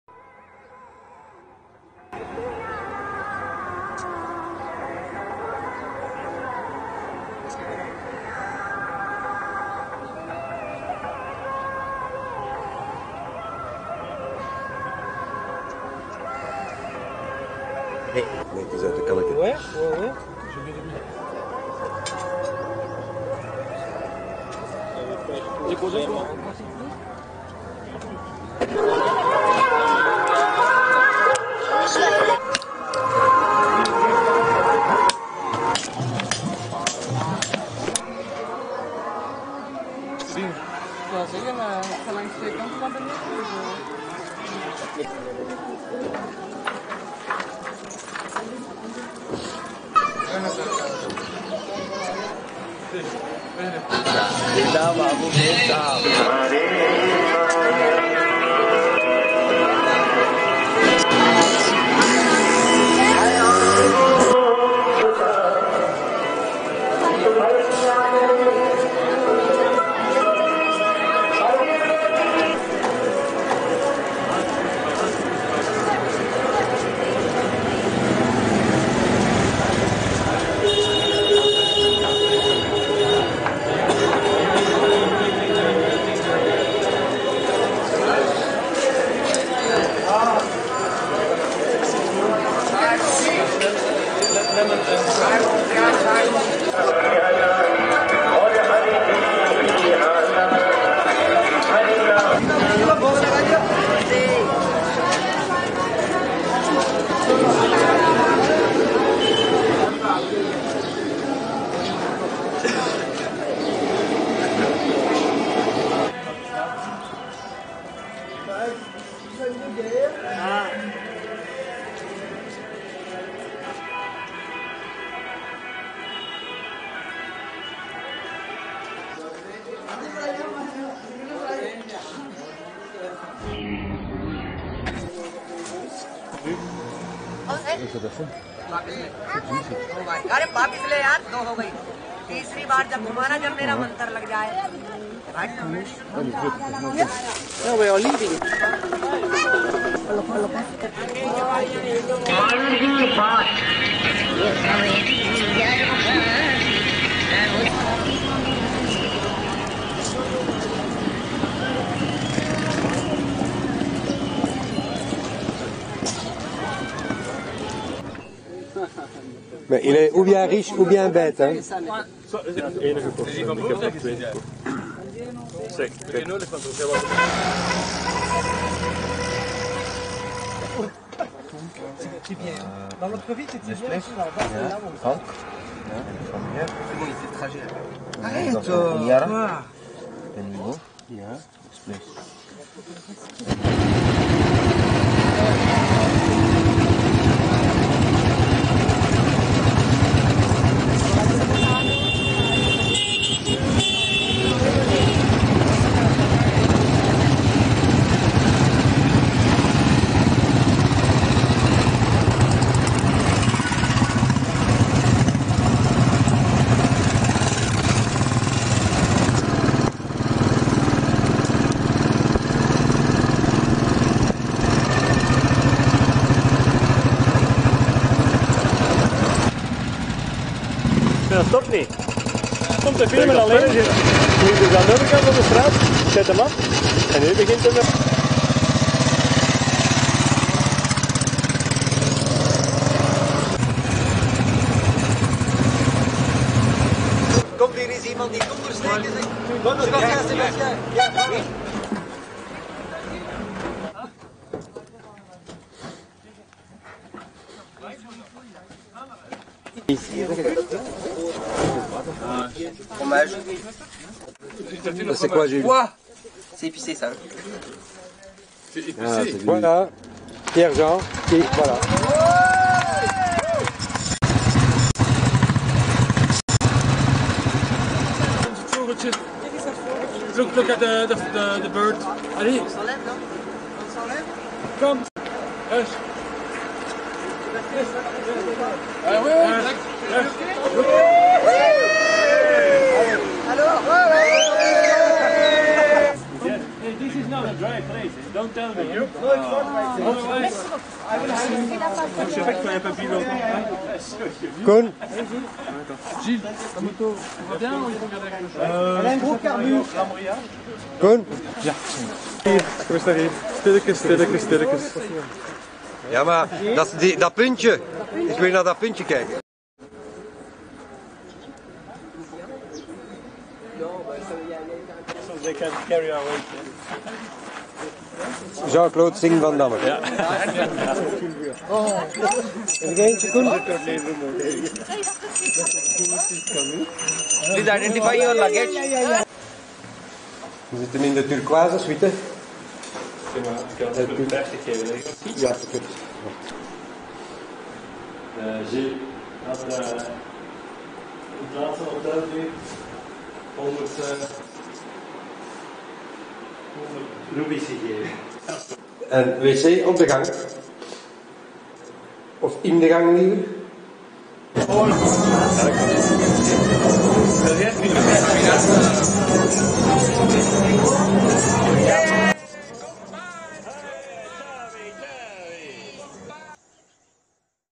011126 Rajasthan day 7
Motorcycletravel to Rajasthan with MotoPulsion in November 2001, day 7. Royal Enfield Bullet from Pushkar to Khimsar. Sundown on the dune.